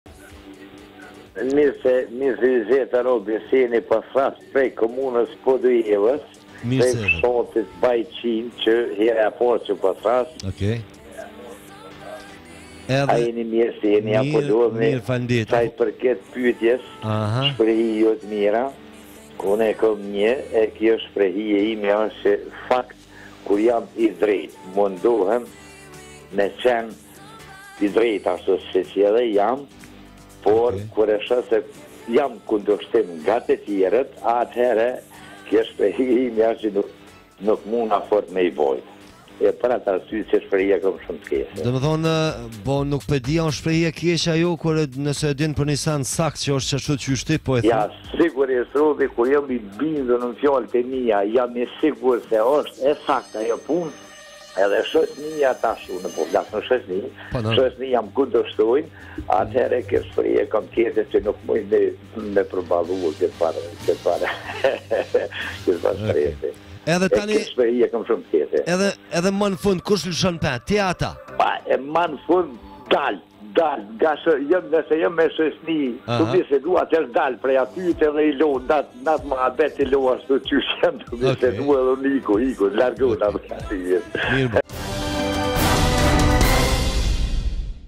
Një dëgjues nga Podujeva “Kur jam i sigurt në fjalët e mia nuk e duroj shoqërinë"
rtv21.tv